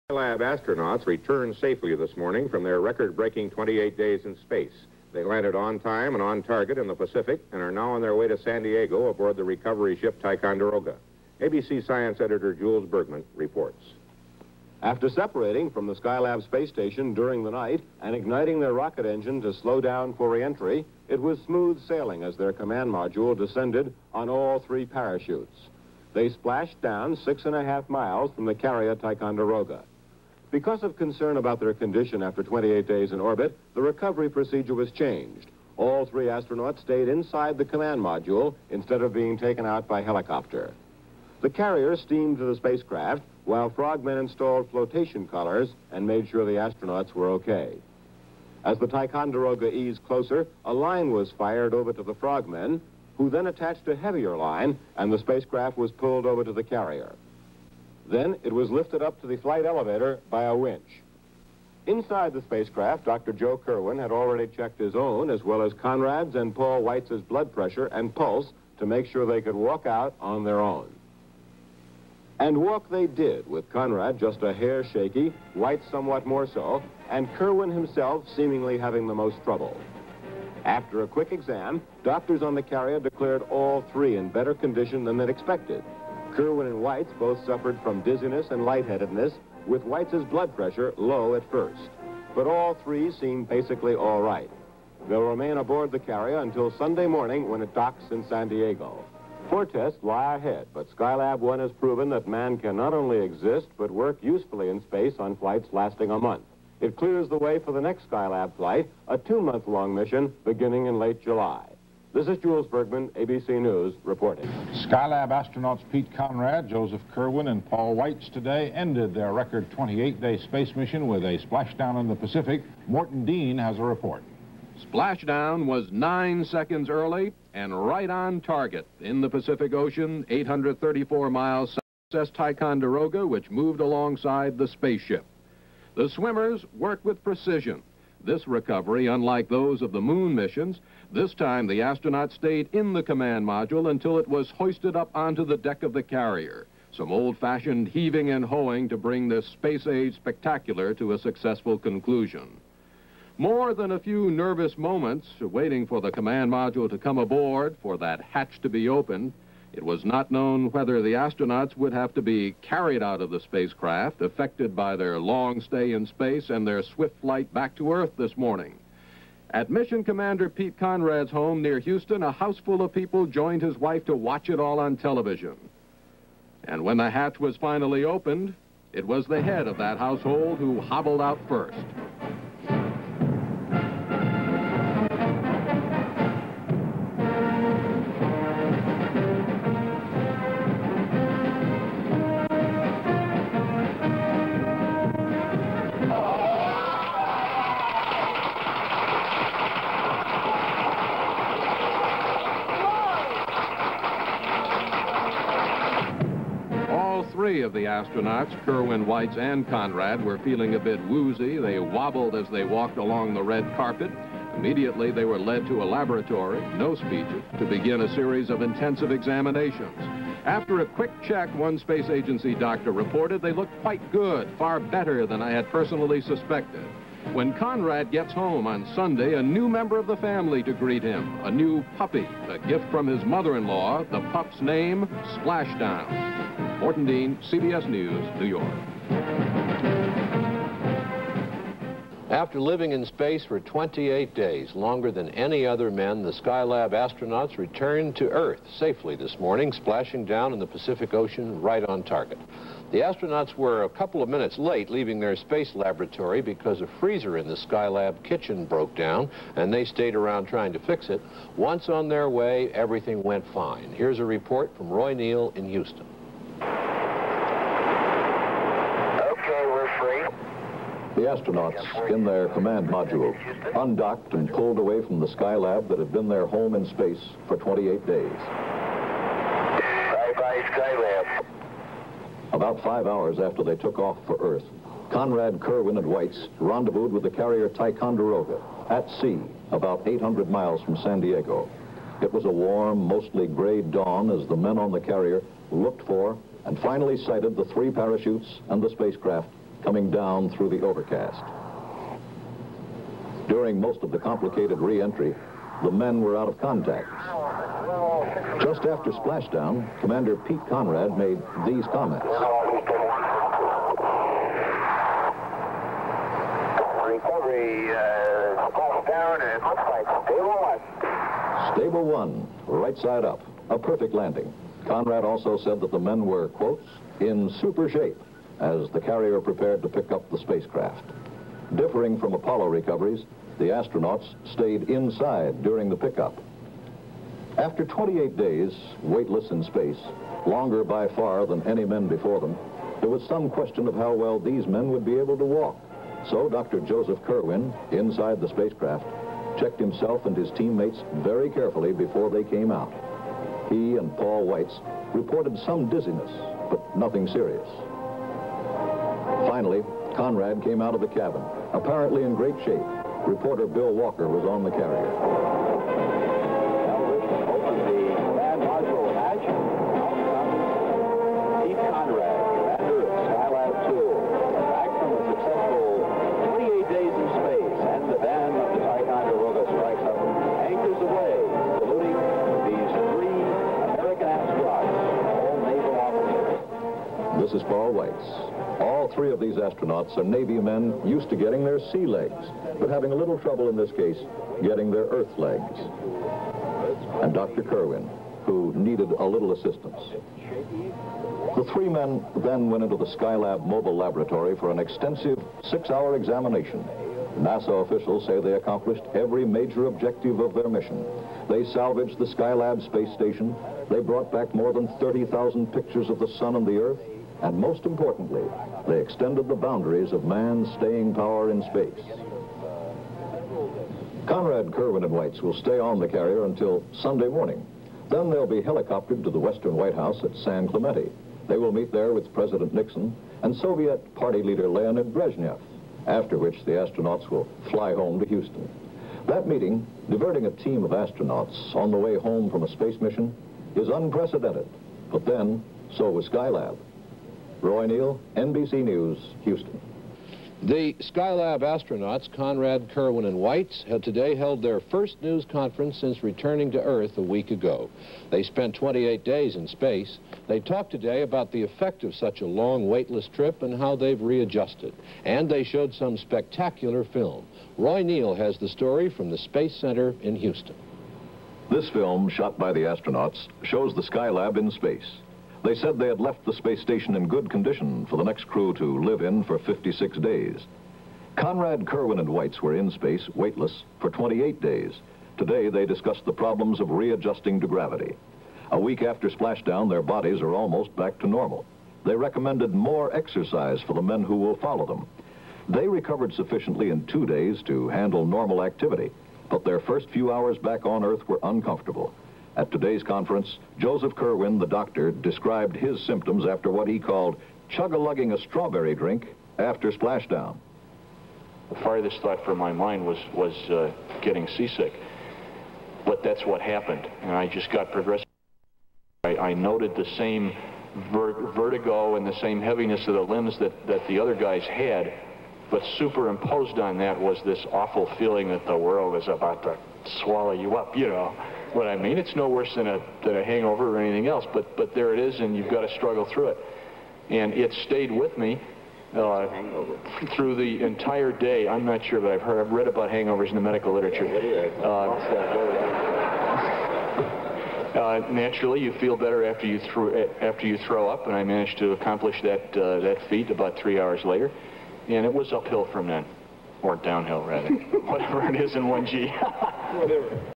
Skylab 2 Part 12 of 12 Evening News Reports
From June 22nd trough June 29th CBS,NBC & ABC News reports of the Skylab 2 mission

The Skylab 2 Crew:

Commander: Charles Conrad, Jr.

Pilot: Paul J. Weitz